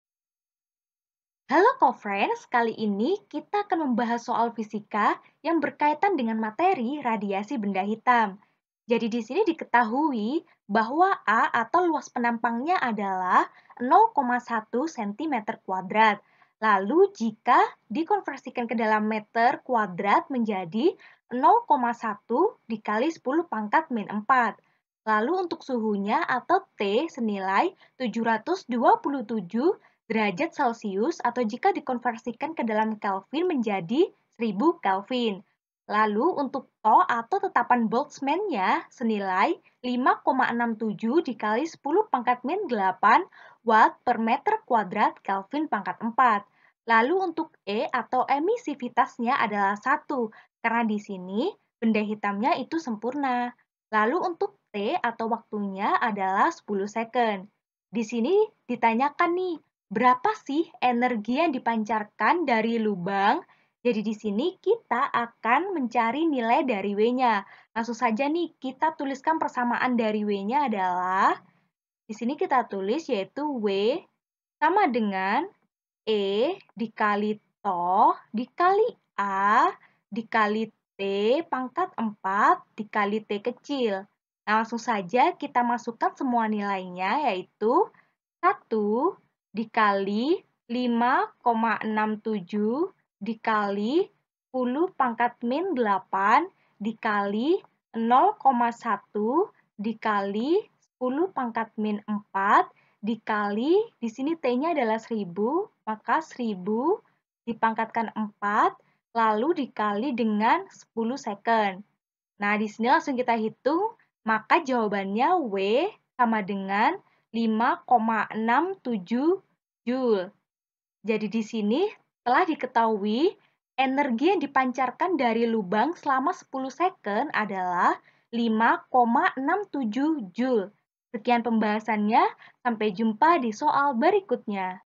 Lubang kecil yang luasnya 0,1 dalam perapian cm? suatu yang suhunya 727*C dapat dianggap sebagai ...
Lubang kecil yang luasnya 0,1 dalam perapian cm? suatu yang suhunya 727*C dapat dianggap sebagai benda hitam sempurna. Hitung energi yang dipancarkan dari lubang selama 10 sekonl

Topik atau Materi: Konsep foton - Konsep dan Fenomena Kuantum - Fisika Quantum - Fisika Kelas 12 SMA -

Sumber Soal: Aktif Belajar Fisika untuk SMA Kelas 12, CR, 2009 , Pusat Perbukuan Departemen Pendidikan Nasional

00:00 : Bedah Soal
01:00 : Konsep, rumus dan pengertian Konsep foton
01:18 : Langkah penyelesaian soal
01:14 : Jawaban akhir
01:44 : Penutup